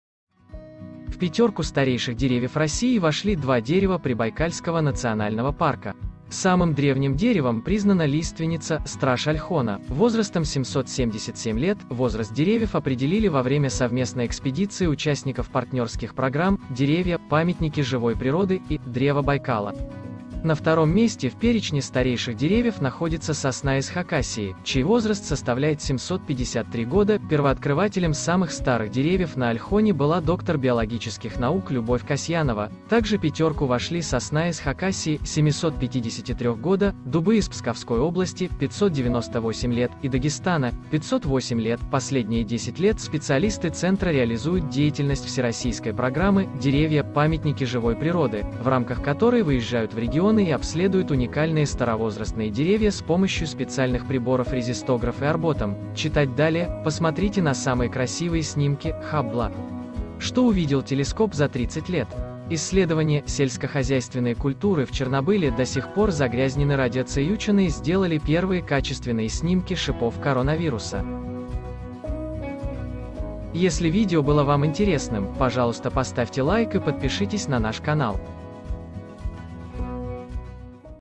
Ученые назвали возраст старейшего российского дерева
Ученые выяснили, что самое старое дерево в России — лиственница, растущая уже 777 лет на острове Ольхон.